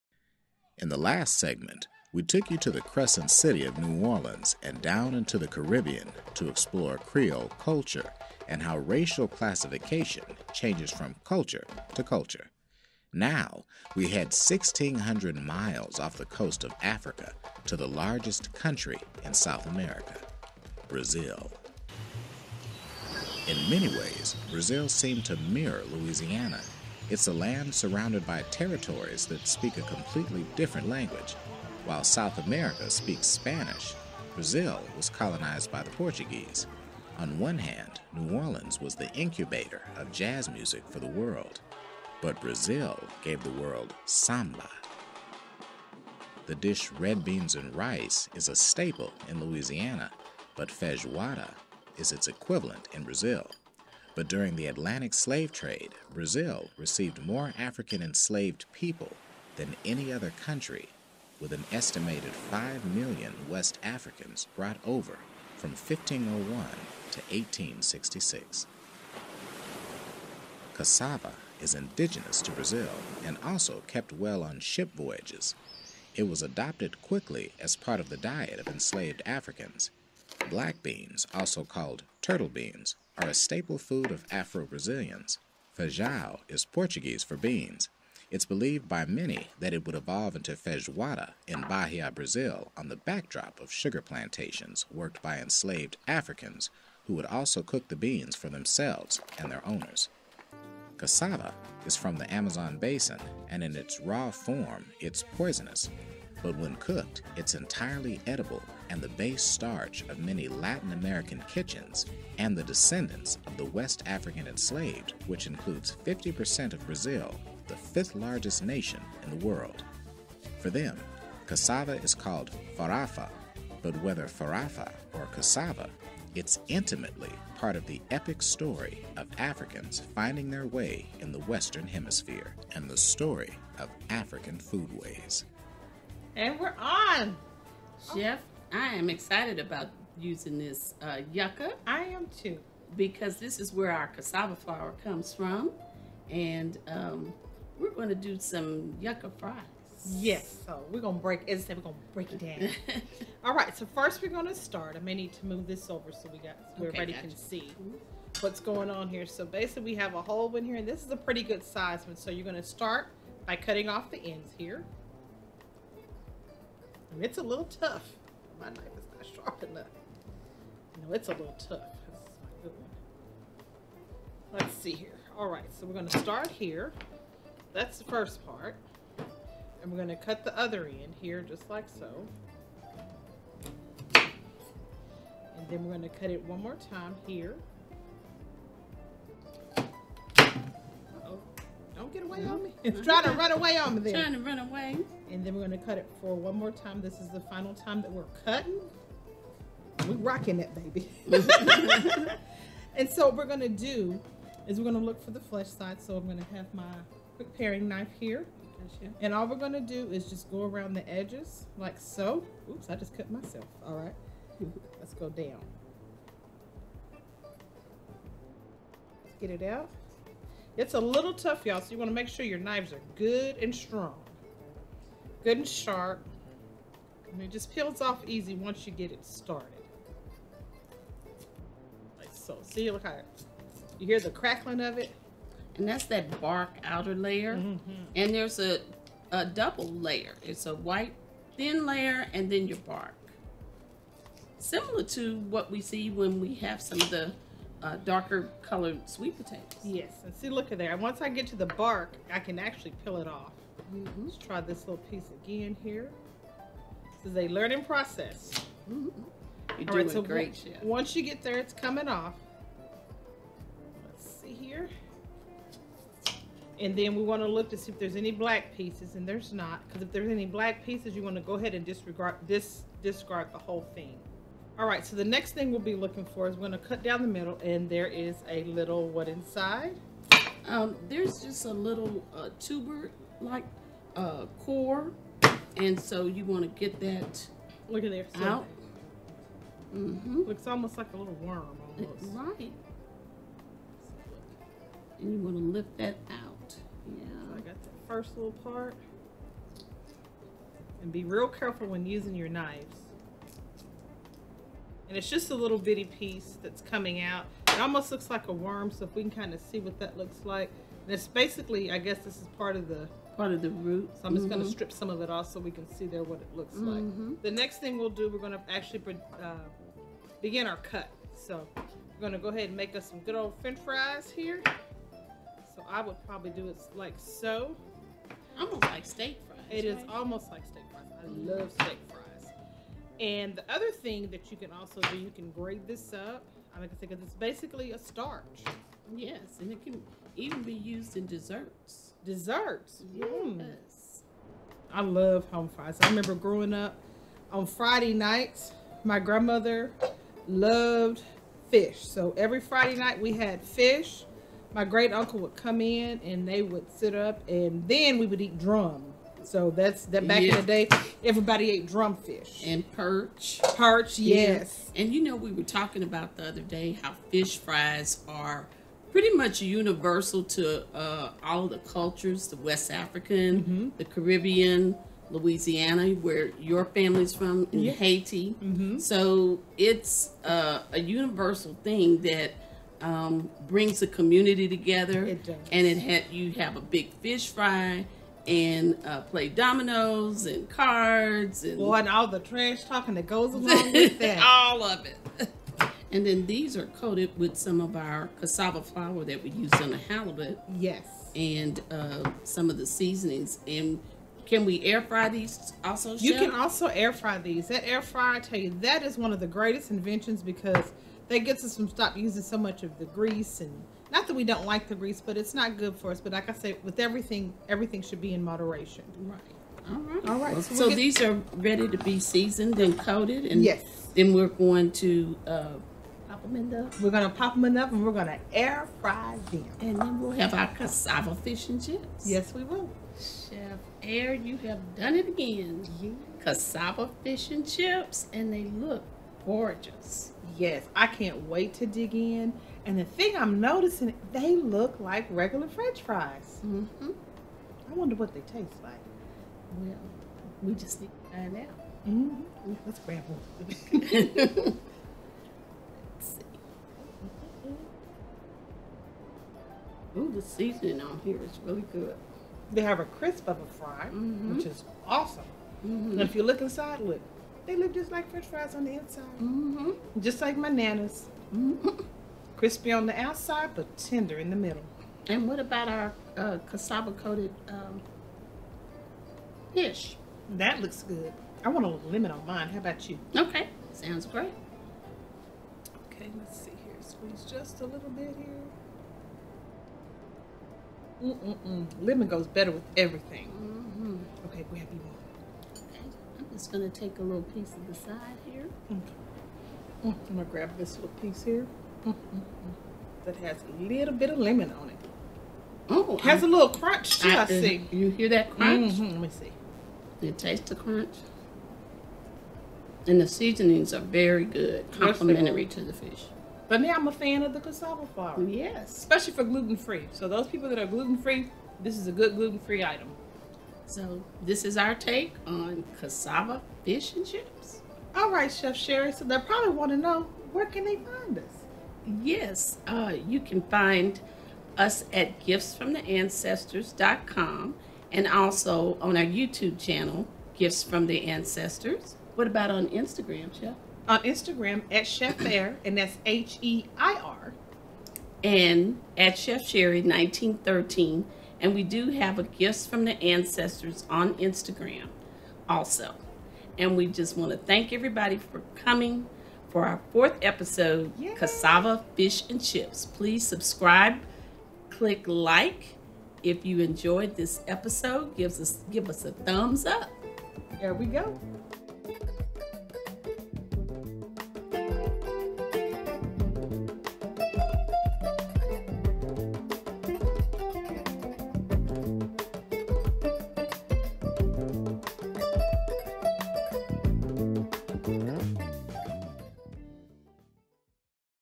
Gifts from the Ancestors Episode 4 - Cassava Fish & Chips
A culinary journey that explores West African Influences on Creole Culture and cuisines in the Heart of Acadiana. Features Chef Sheri L.Raleigh of Lake Charles LA and Chef Natasha Jarmon Heir of Waco, TX. Gifts from the Ancestors is a love letter to our Creole heritage and a testimony of our West African and Caribbean Ancestors. Join us each Sunday in February 2022!

Food Styling: Chef Amiah Yearby and Sous Chef Adriana Adams
Filming and Photography: Chef Amiah Yearby and Hunter Jarmon

Oven-Fried Halibut
Adapted from Leila Ali, Home Made Simple

Serves: 2 to 4 people
Prep time: 20 minutes
Cook time: 15 minutes
Difficulty: EasyIngredient

Ingredients
1 tablespoon Soulful Seasoning Salt
1 teaspoon chipotle powder
1 pound halibut, cut into 8 fillets
Avocado oil spray

Crispy Oven-Fried Halibut
1. Cover a baking sheet with parchment paper. Spray avocado oil to heavily coat the parchment paper
2. In a shallow bowl, combine the flour, Creole Seasoning Blend, and chipotle powder. Add salt to taste. Pat each fish fillet dry and lightly dredge in the cassava flour mixture. Shake off excess flour
3. Place the fish on the coated parchment. Generously spray the top of each piece of fish with the avocado oil
4. Bake the fillets at 450 F for 5 minutes. Lower the oven temperature to 400 F and continue to cook for an additional 10 minutes or until the fish is opaque throughout.

Baked Yuca Fries
Baked Crispy Cassava Fries (Yuca Fries) - crunchy, slightly sweet baked cassava fries that make a great healthy alternative to the usual French fries. Simply guilt-free snacks!

Servings 3 -4 people
Calories 522 kcal

Ingredients
2-3 pounds of cassava
4 garlic cloves sliced
2-3 tablespoons sugar
pinch of salt
3 tablespoons of chopped parsley
Cooking spray or canola oil

Instructions
1. Cut off both ends of cassava and then cut them in half.
2. Make a shallow cut into the skin of the cassava and gradually work your knife under the layer of the cassava to remove the skin.
3. Cut them into desired shapes, rinse the cassava, and place in a pot with salt, sugar, and water, just enough to barely engulf the cassava.
4. Bring to a boil and cook on medium heat until fork tender. Do not overcook the cassava. Drain the water from the cooked cassava.
5. Line a baking sheet with foil or parchment paper and place the cassava in a single layer.
6. Sprinkle with garlic and parsley and coat the cassava with cooking spray or oil
7. Bake in a 425-degree oven for minutes for 20 minutes or until slightly brown, turning once
Serve warm with hot sauce or aioli